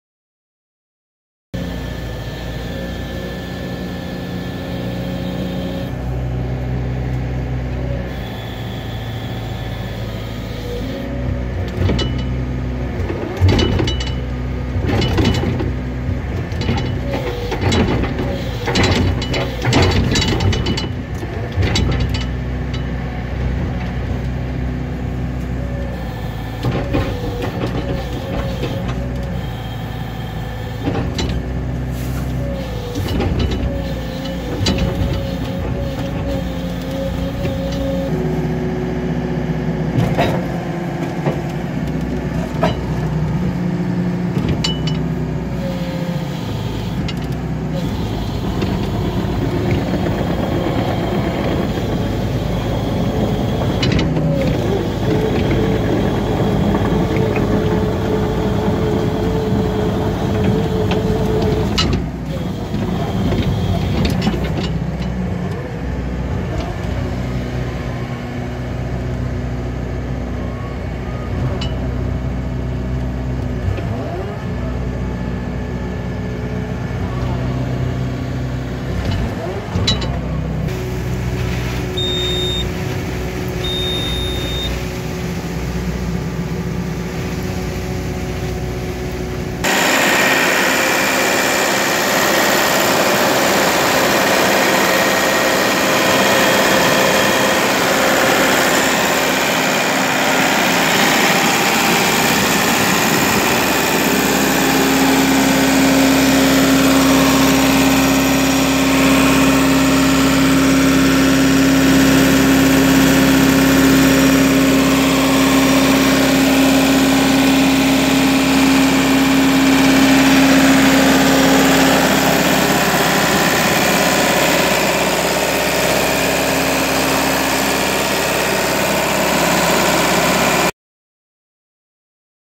Used 2007 - BOBCAT 442 B for sale | Used Mini-Excavator | equippo.com
FULLY INSPECTED MACHINES - SAFE PAYMENTS - EASY INTERNATIONAL LOGISTICS

Video of a USED 2007 Bobcat 442 B Midi Excavator with 2 buckets, quick coupler, hammer lines and air conditioning.